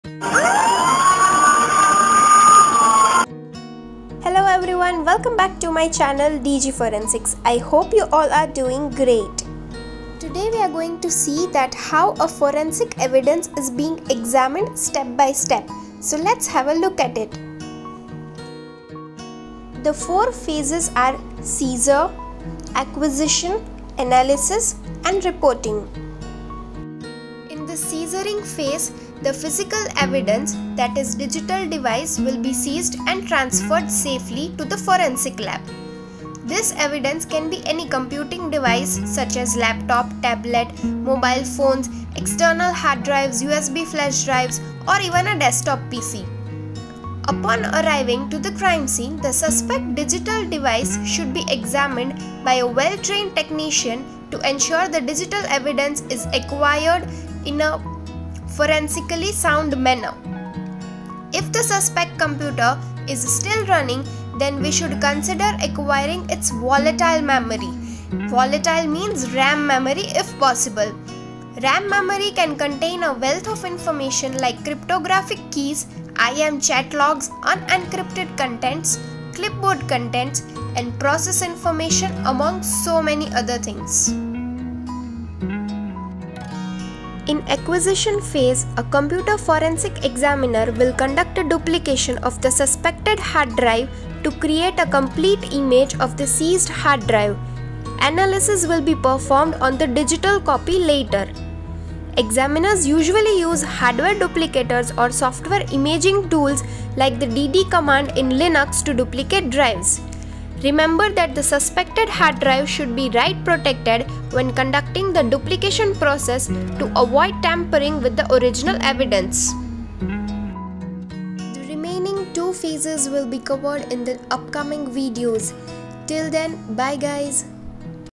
Digital Forensic Evidence Examination Process [PART - 1]
This video will teach you in detail that how an evidence is being examined by the forensic experts.
All the phases of examination will be discussed in depth with you all.